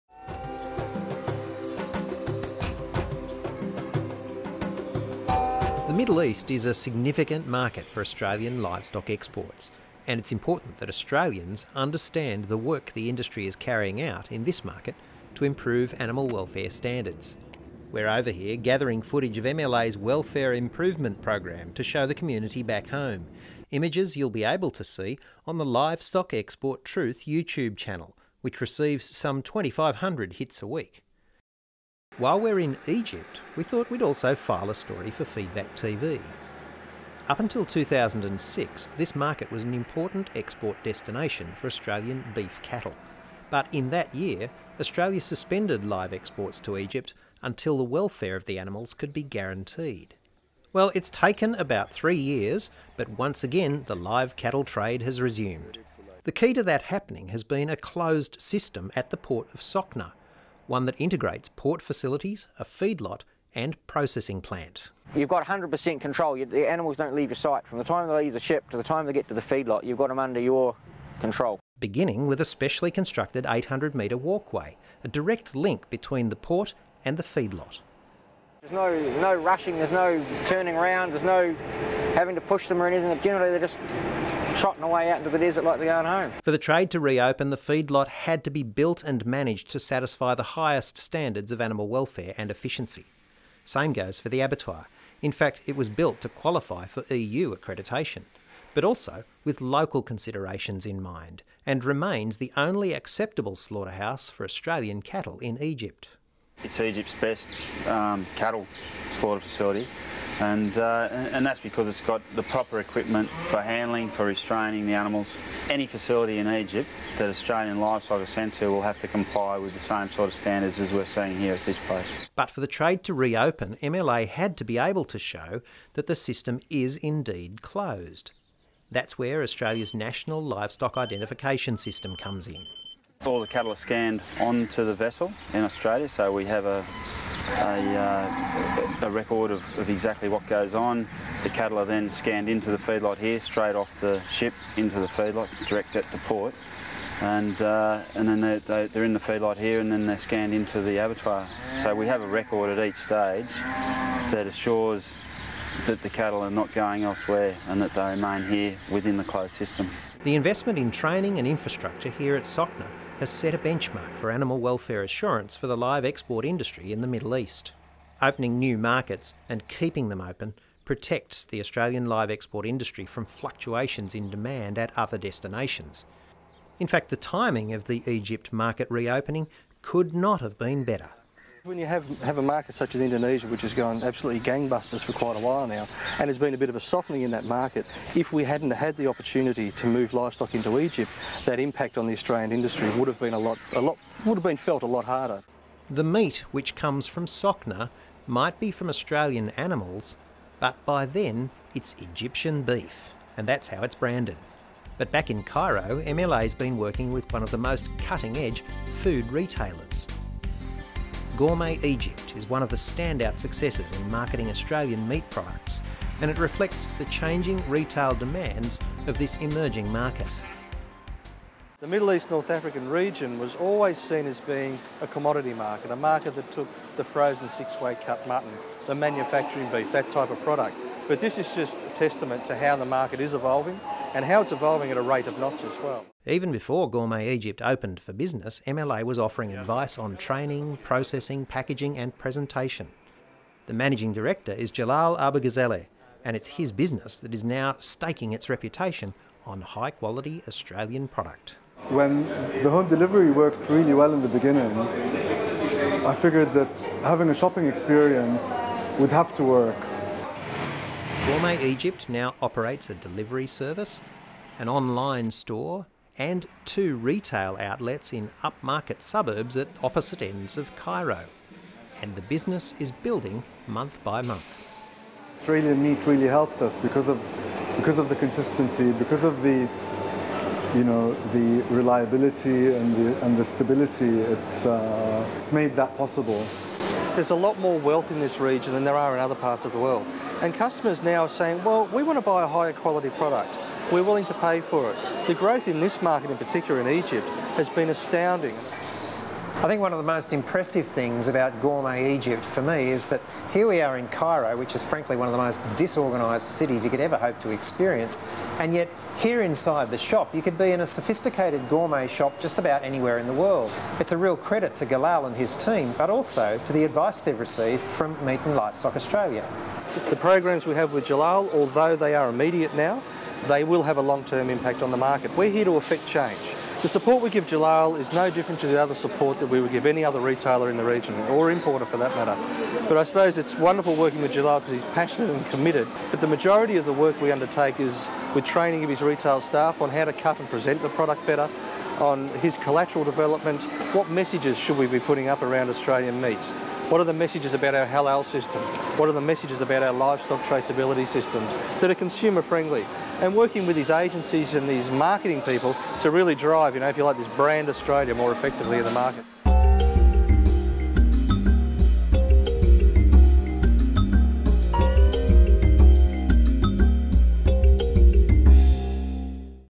Meating all of Egypt's needs (Episode 14)
Learn about Egypt's increasing demand for Australian beef and lamb boosted by MLA's marketing and training programs for retailers.